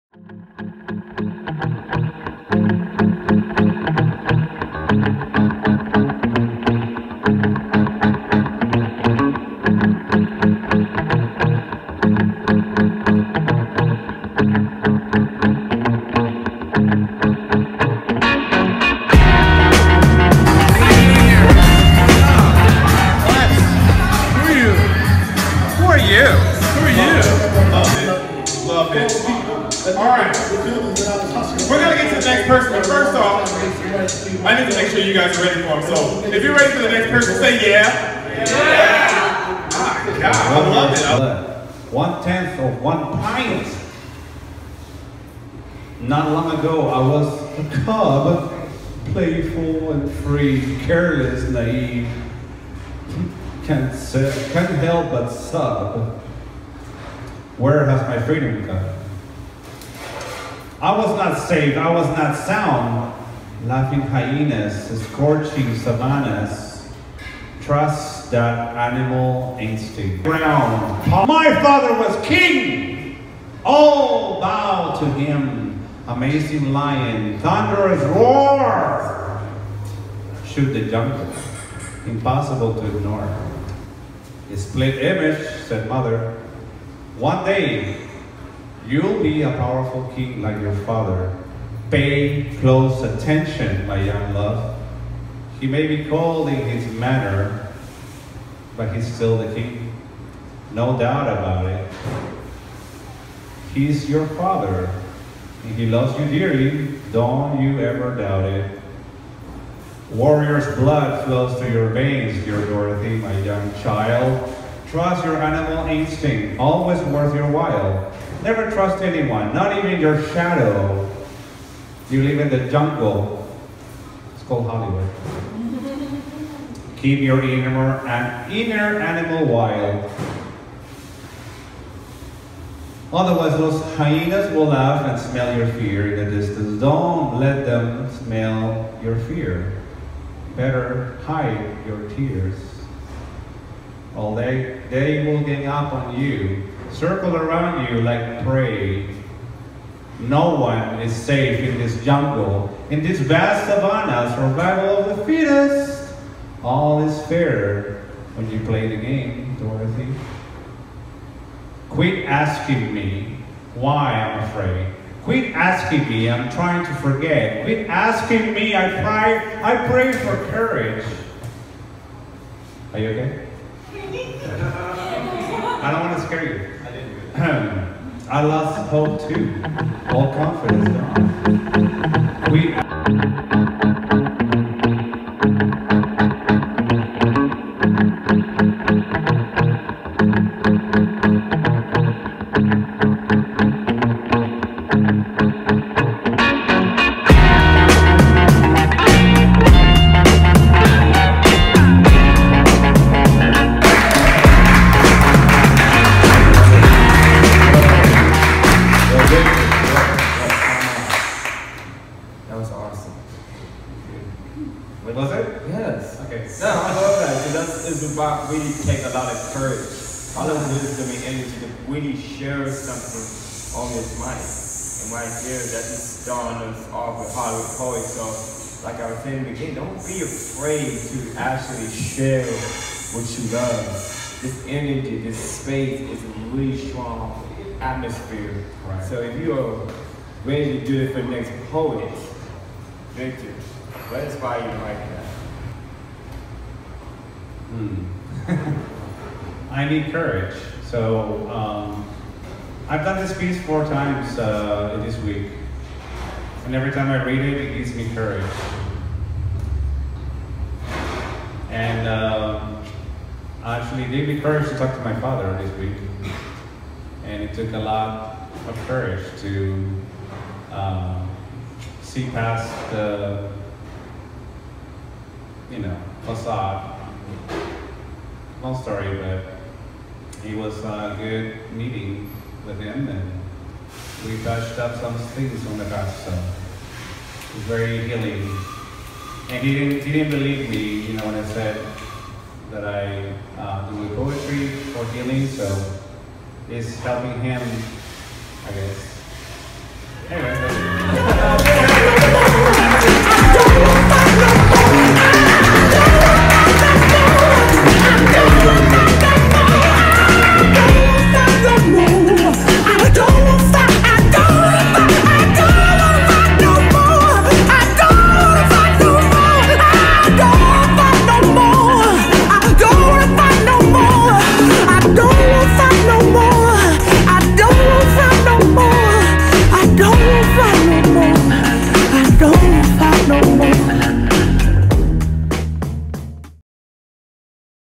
Warrior's Blood at Hollywood Poets
Hollywood Poets is hosted by Ideas Aubrey and TranSe Carter every Saturday night at 7:00PM at Catch 56 in Hollywood, come check it out. Here I performed Cowardly Lion's Warrior's Blood